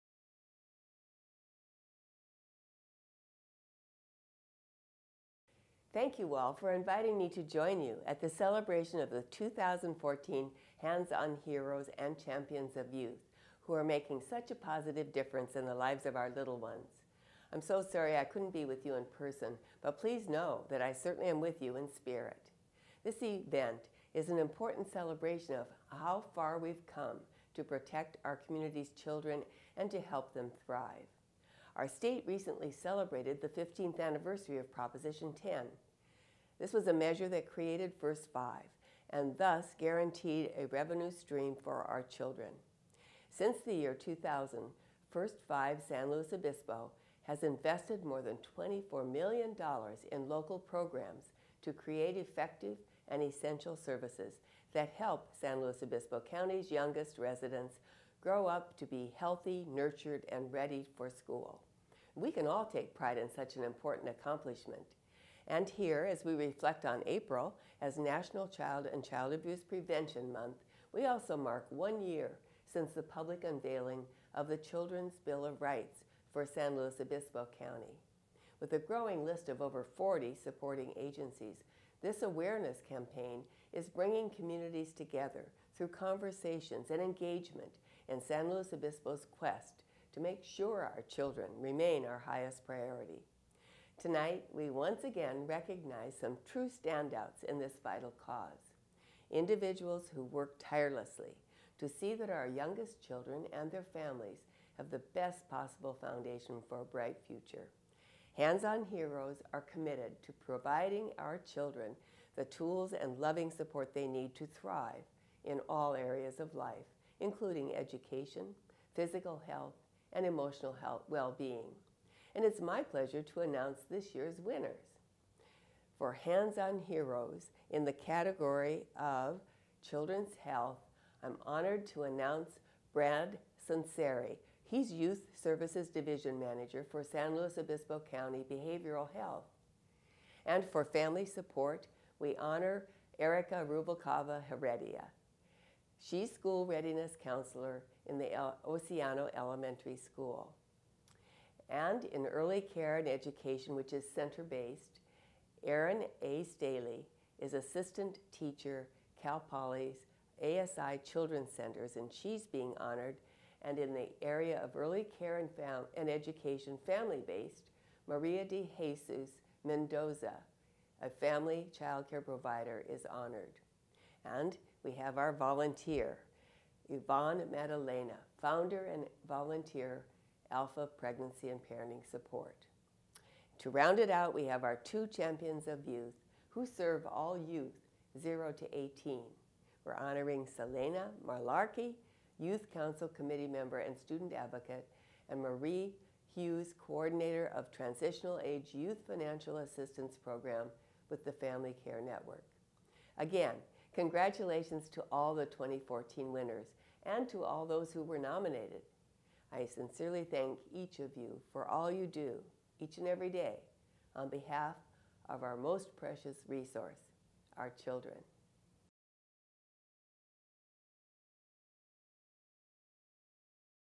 Congresswoman Lois Capps honors 2014 Hands-on Heroes
Congresswoman Lois Capps addresses annual dinner to honor 2014 Hands-on Heroes - San Luis Obispo, CA